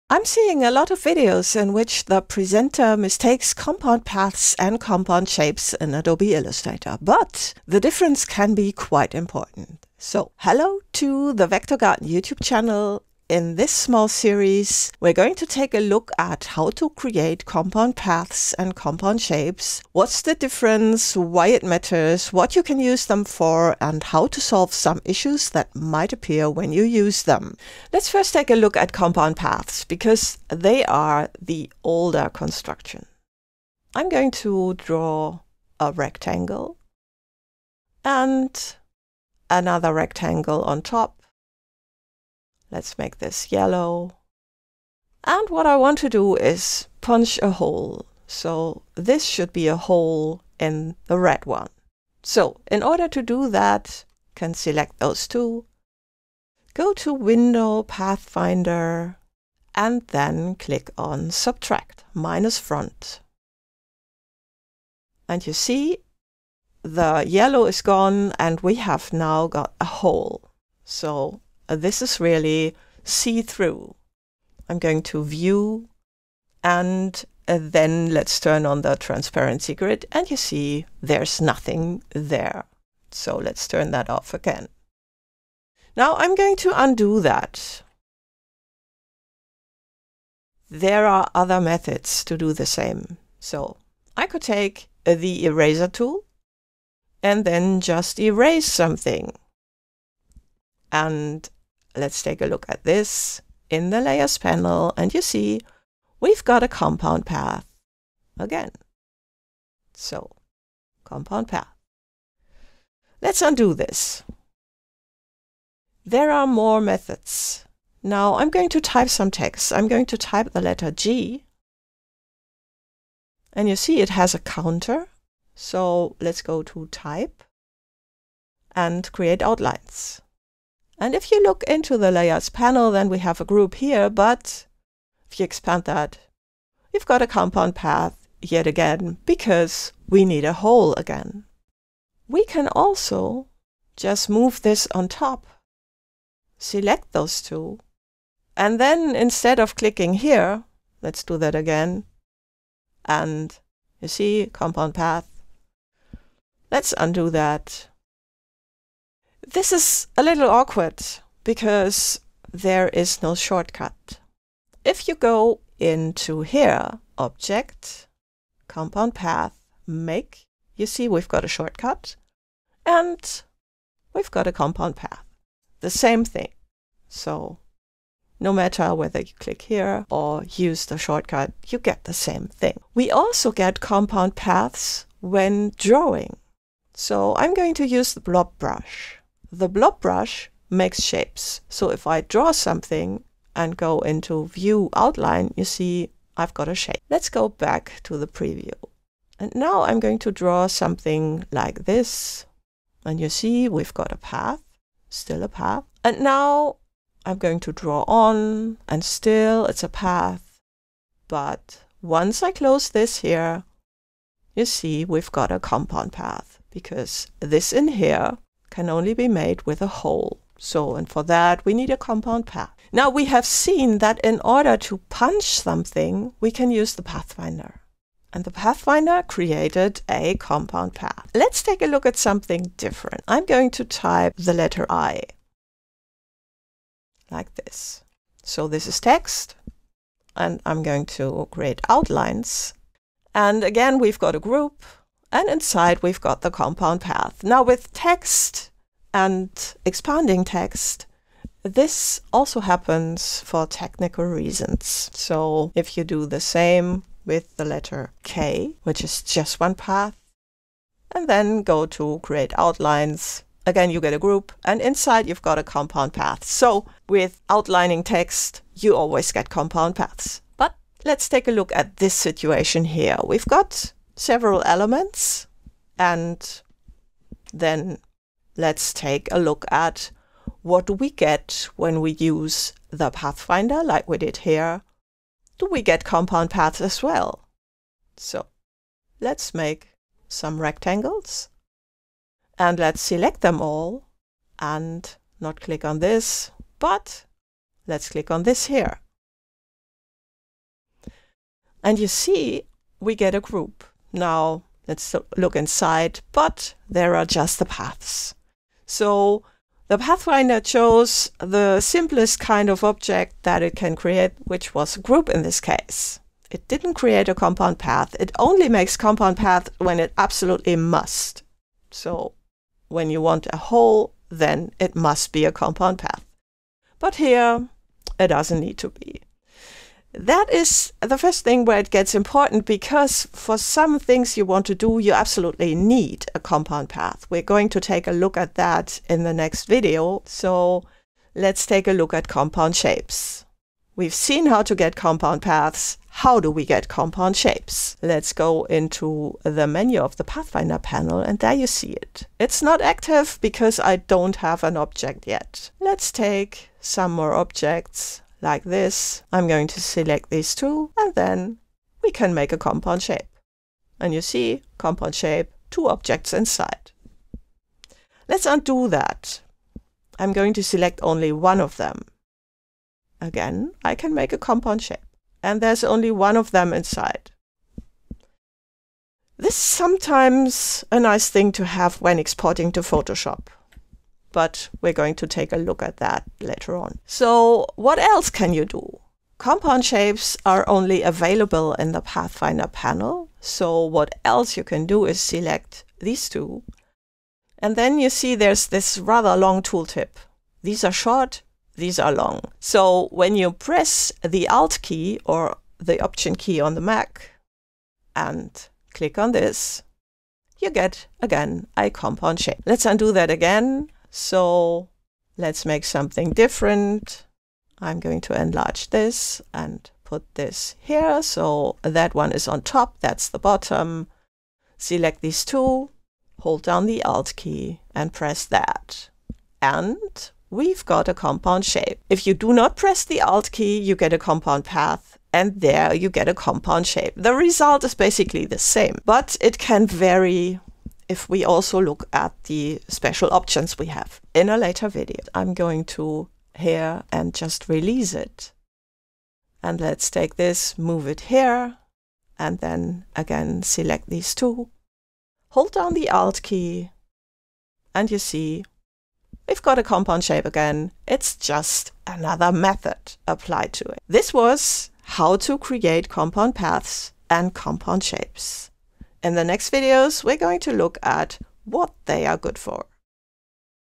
Understanding Compound Paths And Compound Shapes
When you mistake compound paths for compound shapes or aren't even aware that both exist, this can fall onto your feet in your workflow. And since most tutorials don't even get the terminology
correct, it's even hard to find help in such moments.
During the next few weeks this series takes you through the concept.